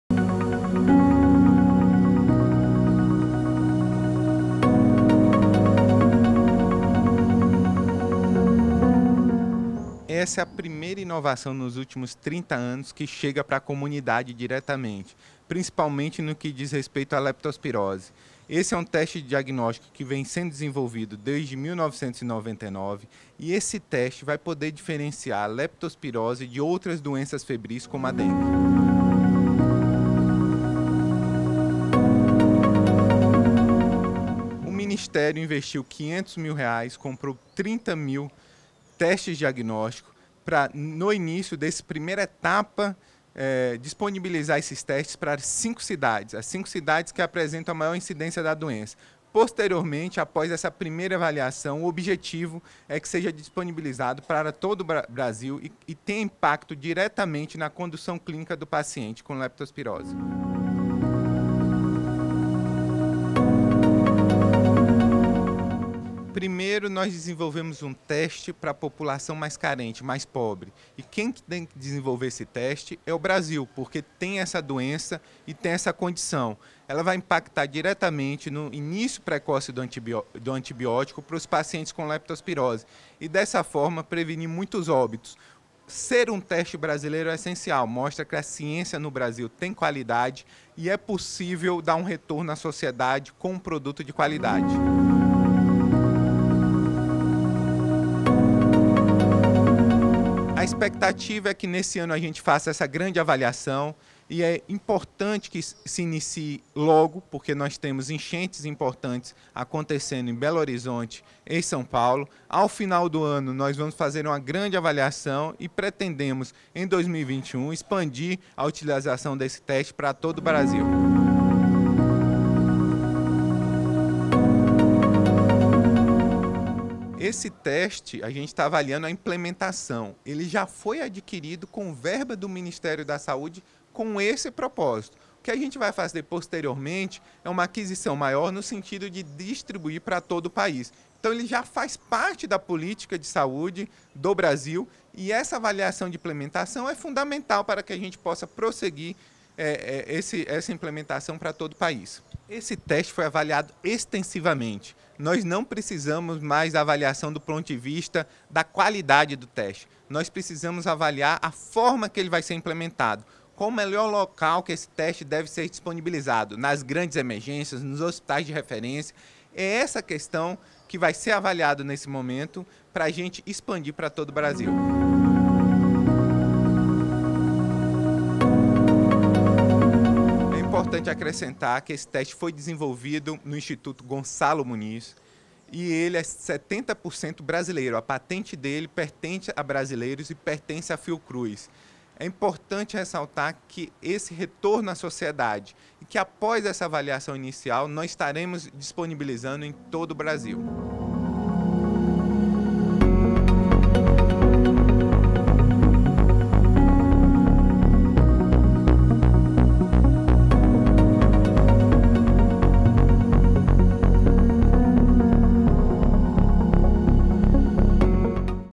Lançamento do teste rápido para diagnóstico de leptospirose
O Ministério da Saúde e a Fiocruz Bahia lançaram, no dia 13/02/2020, o teste rápido para diagnóstico de leptospirose. O kit diagnóstico foi desenvolvido por pesquisadores da Fiocruz Bahia a partir da parceria com Bio-Manguinhos/Fiocruz, unidade produtora de imunobiológicos Fiocruz.

O novo teste apresenta resultado em até 20 minutos e será executado em ambiente hospitalar. Um projeto para avaliar a implantação do teste deverá contar com participantes de todas as regiões do país.